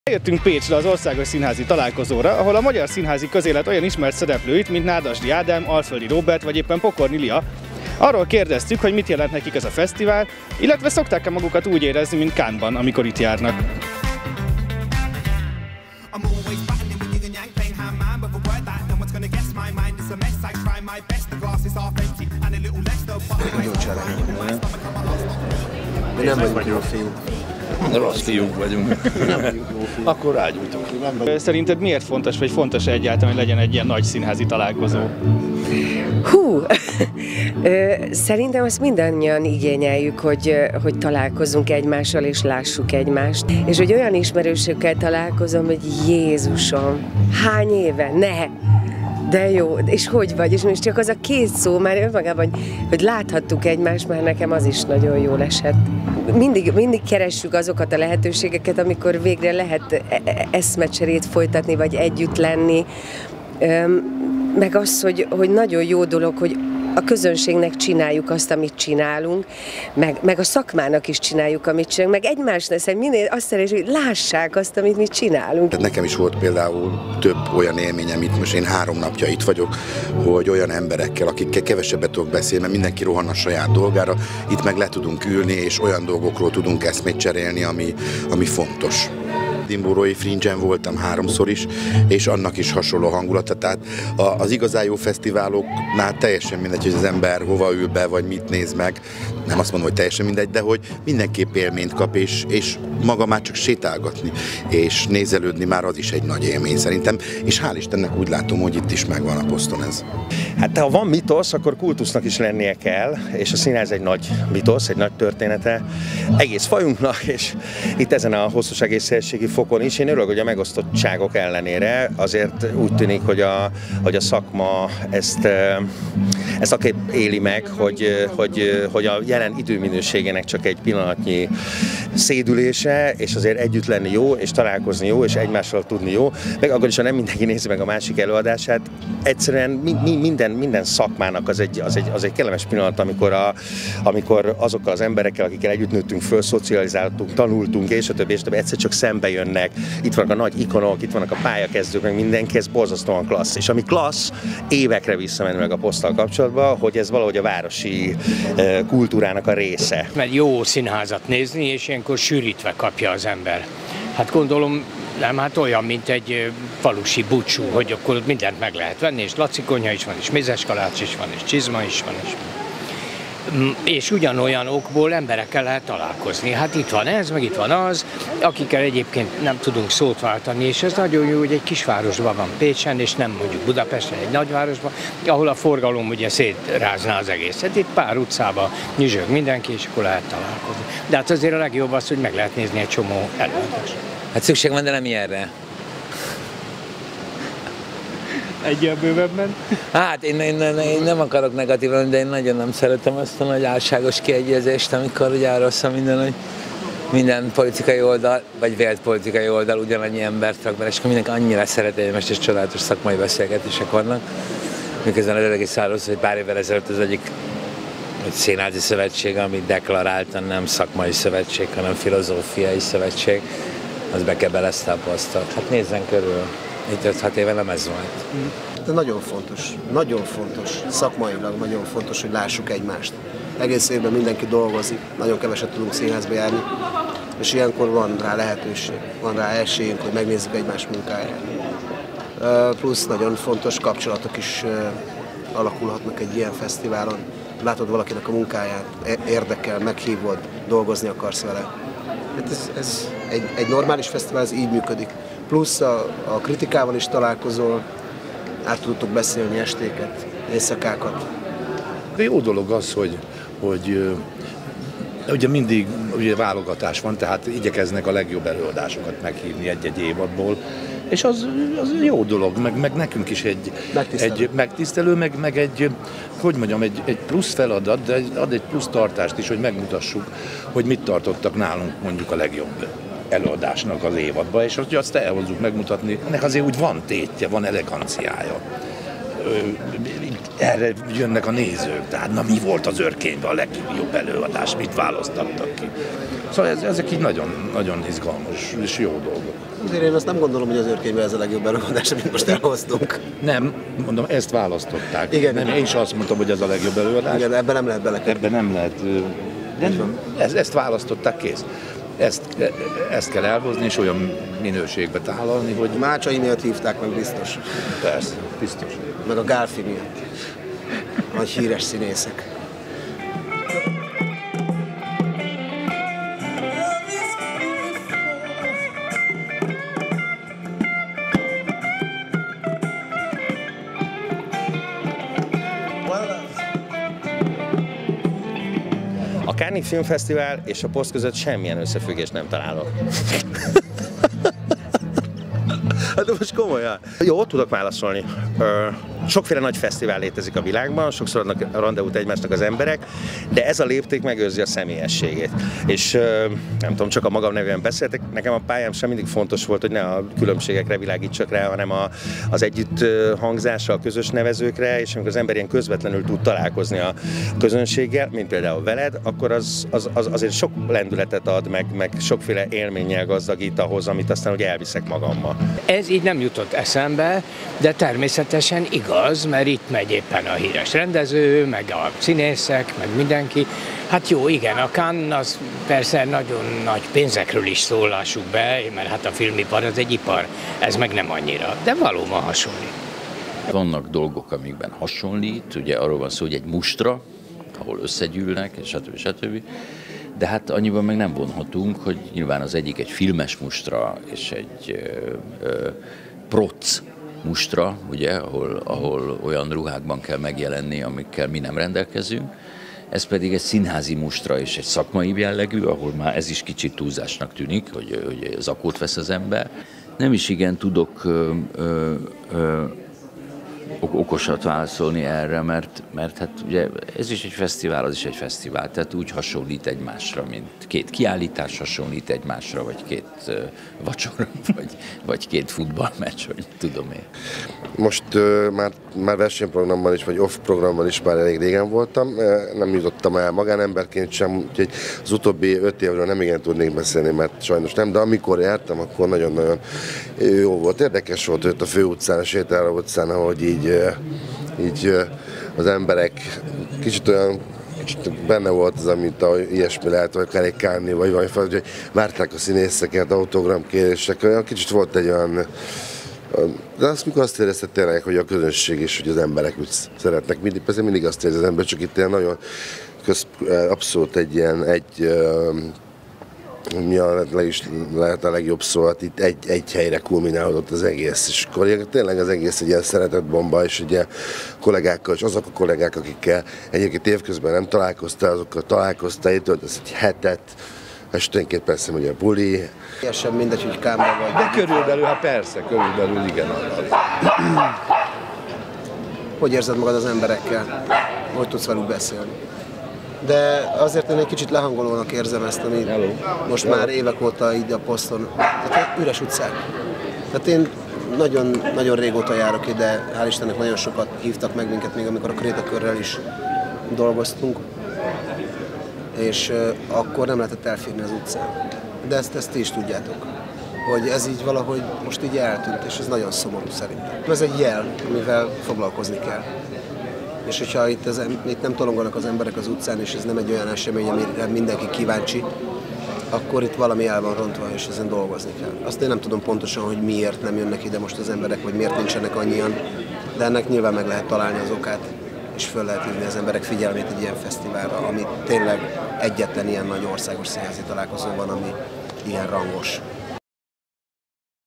POSZT – a magyar Cannes?
Hideget és meleget egyaránt kapott az ország legnagyobb színházi találkozója azoktól az alkotóktól, akikkel a fesztivál két fő helyszínén, a Pécsi Nemzeti Színház körül, illetve a Zsolnay Negyedben találkoztunk. A felvételen megszólal többek között Alföldi Róbert, Pokorny Lia, Mácsai Pál, Novák Péter és Nádasdy Ádám is.